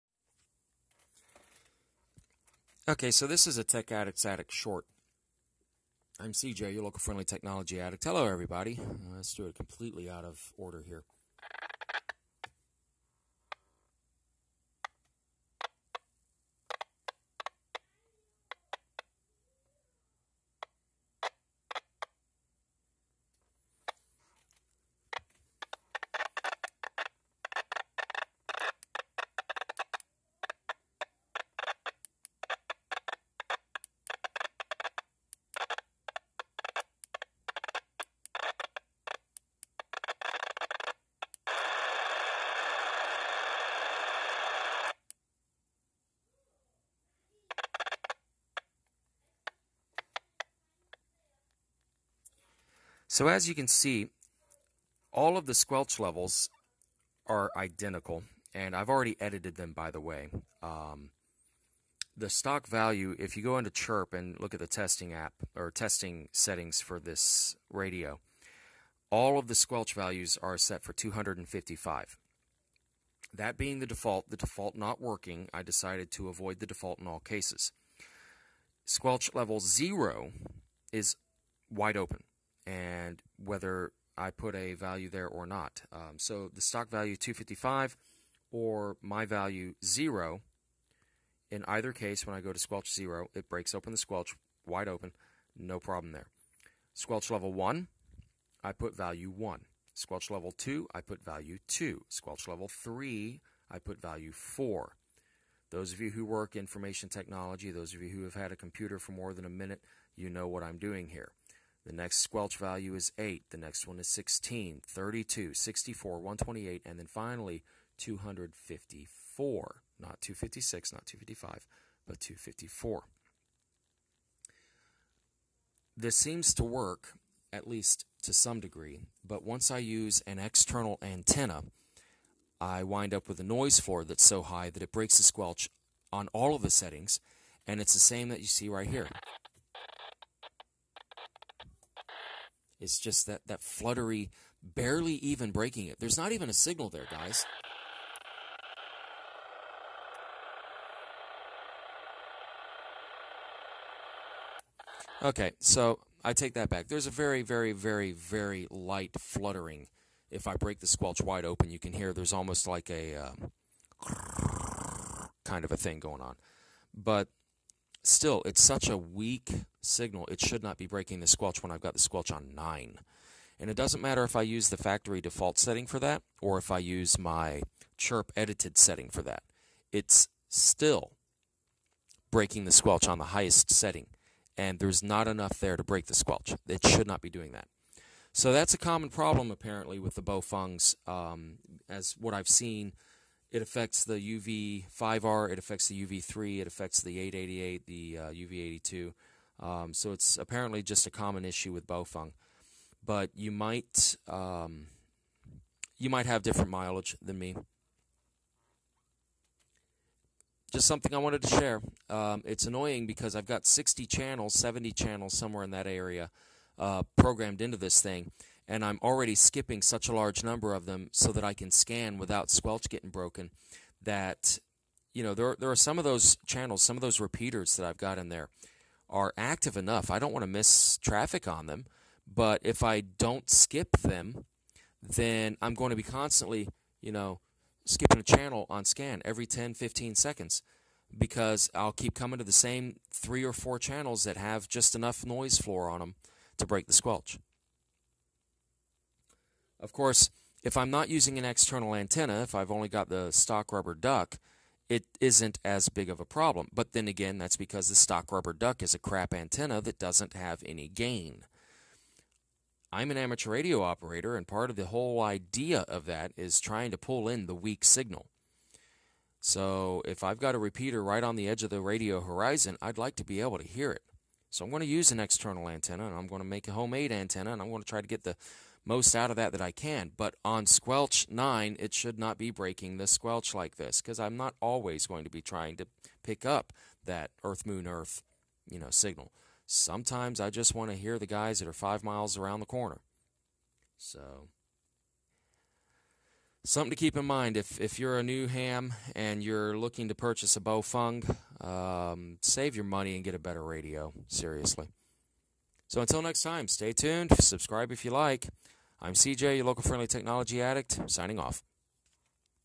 Example of Baofeng Squelch Problem
Even level 9 squelch isn't enough to block out random noise floor ridges.